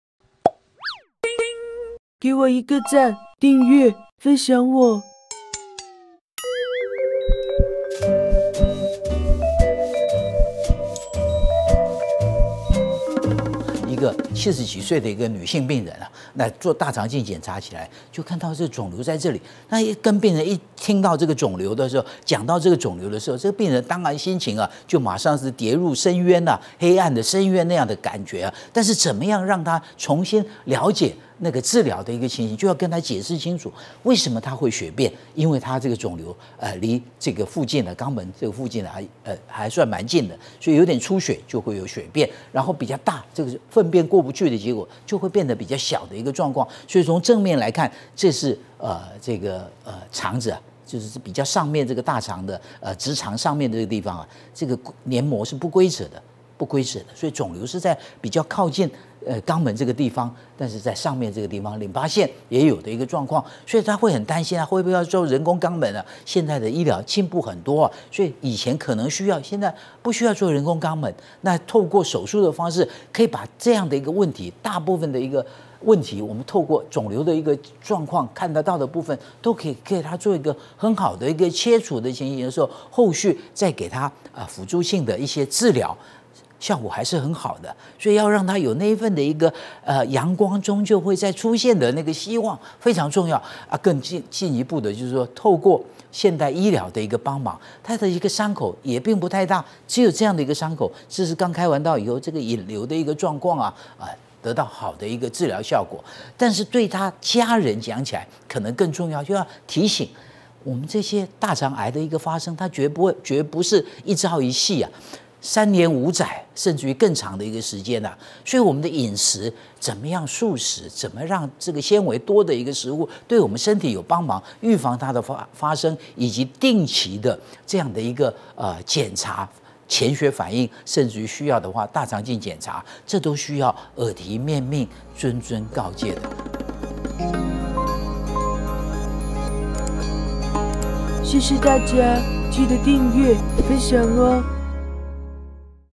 【上廁所流血🚽 血便會是腸癌嗎?😱】【💝大愛醫生館🏥精選】
上廁所流血🚽   血便會是腸癌嗎?😱

七十多歲的老婦，👵
上廁所時出現了血便💉😱。
經過大腸鏡檢查，👨⚕️🩺
果然在肛門附近發現了腫瘤。
老婦人心情跌入萬丈深淵👻，
深怕自己是得了腸癌！😨
要如何利用現代醫療，🏥
幫老婦人做最好的治療👍。
來聽聽簡院長怎麼說？👨⚕️